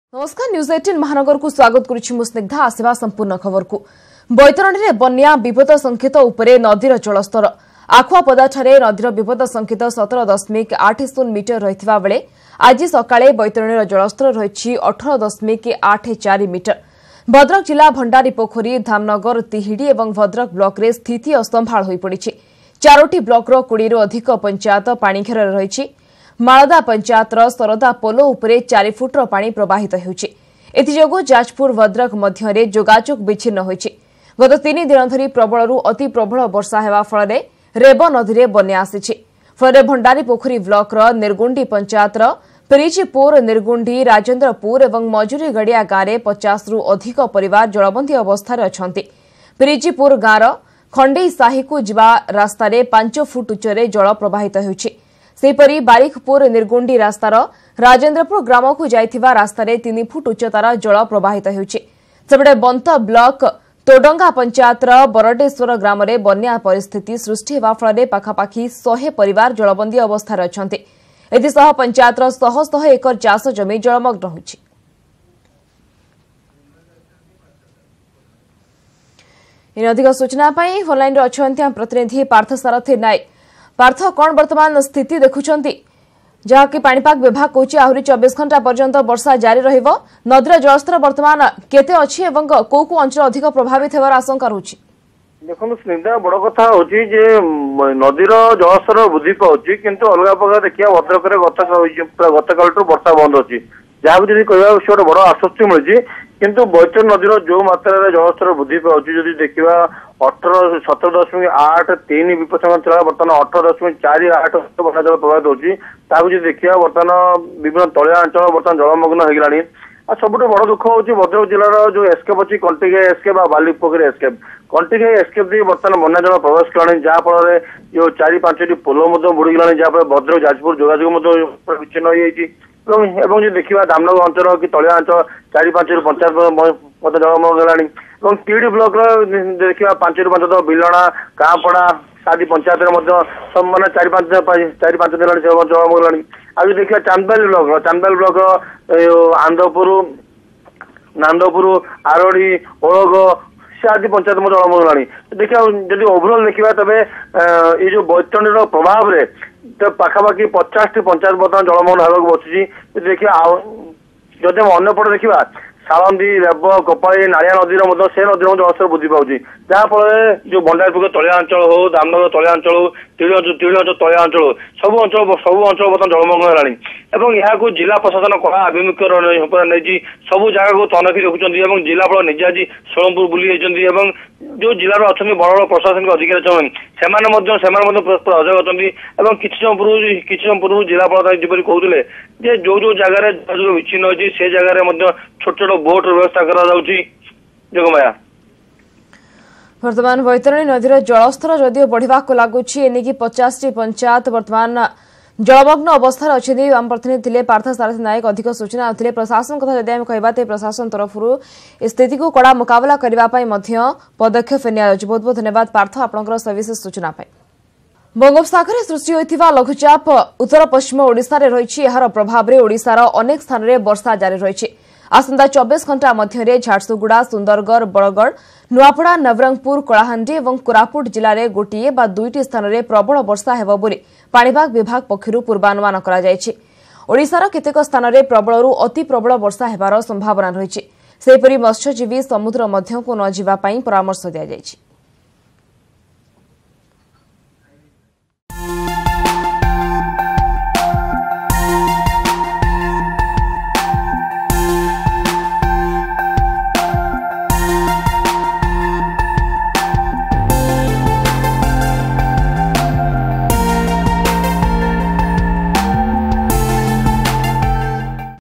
ODISHA FLOOD NEWS | ବିପଦ ସଂକେତ ନିକଟରେ ବୈତରଣୀ | News18 Odia
The baitarani river has crossed the flood like situation in odisha.